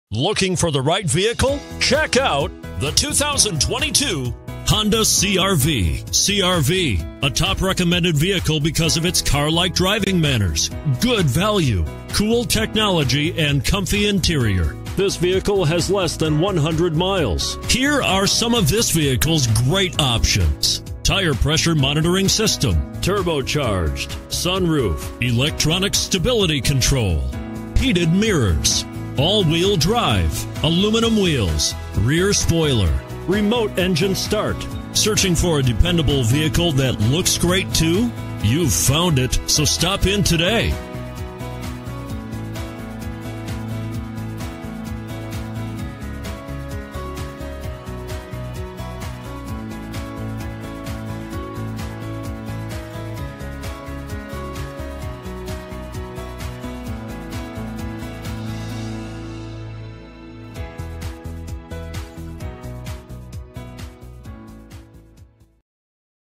2022 Honda CR-V Nanuet NY NH618340
2022 Honda CR-V EX AWD

Obsidian Blue Pearl 2022 Honda CR-V EX AWD CVT 1.5L I4 DOHC 16VbrbrRecent Arrival! 27/32 City/Highway MPG